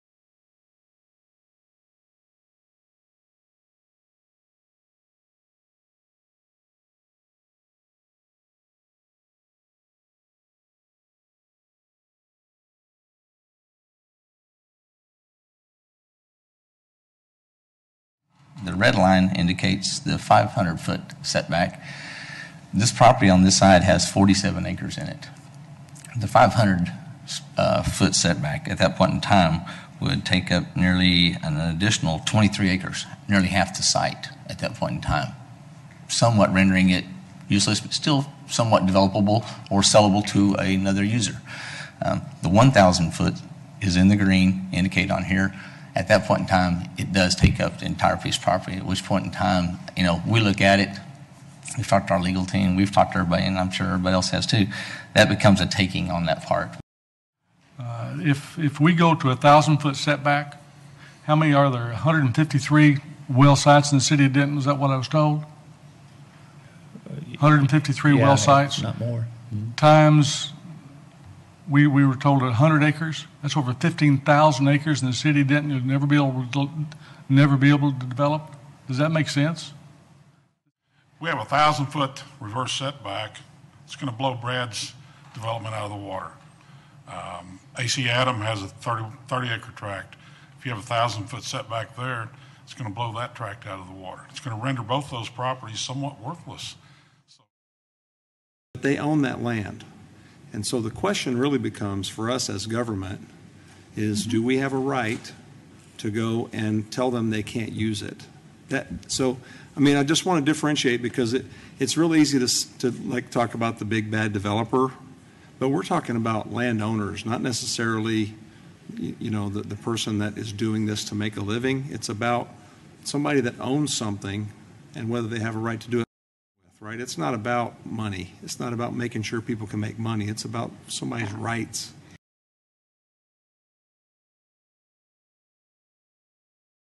Texas Landowners: Extreme Drilling Setbacks Destroy Private Property Rights
Texans speak out against restrictive rules that not only prevent fracking, but also block construction of new homes and businesses. Sign up to defend Texans' property rights and energy independence